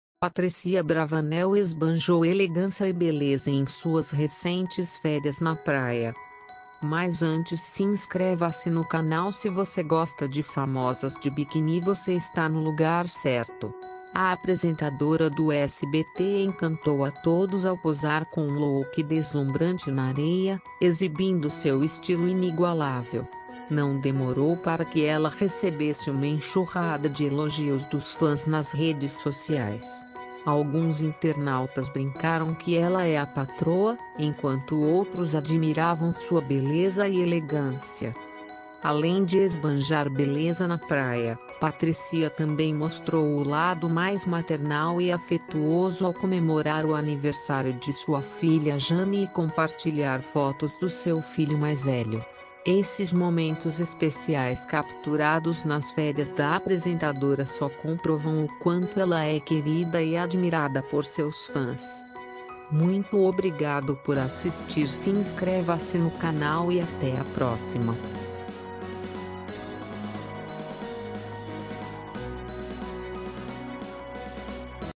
😱FAMOSA DE PATRÍCIA ABRAVANEL, MOSTRA PACOTÃO
FAMOSA DE PATRÍCIAABRAVANEL, MOSTRA PACOTÃO

No vídeo de hoje, a famosa de biquíni Patrícia abravanel, deixa os fãs agitados.

Patrícia Abravanel Faria é uma apresentadora de televisão e empresária brasileira. Ela é filha do apresentador e empresário Silvio Santos, e da novelista e empresária Íris Abravanel. Tornou-se mais conhecida por apresentar os programas Cante se Puder e Máquina da Fama, transmitidos pelo SBT.